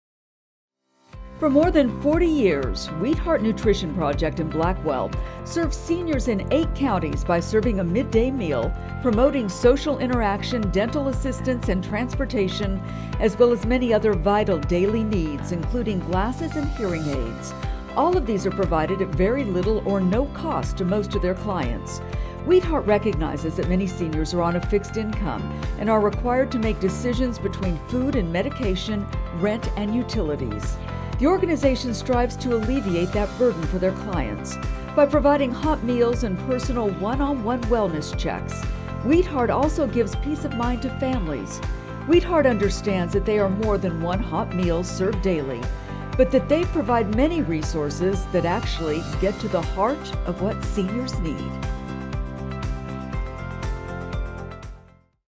ONE Awards 2017 - Wheatheart Nutrition, Blackwell
The Anne & Henry Zarrow Foundation and Herman Kaiser Foundation Award for Seniors Winner

The ONE Awards honors a total of 24 organizations in eight categories: arts and humanities, community, education, health services, open services, self-sufficiency, seniors, and youth development. Additionally, the Center names an overall winner of the event named after Charles Schusterman. All 24 nonprofits receive at least a $5,000 prize, and category winners receive $7,500. The overall winner receives a grand prize of $10,000.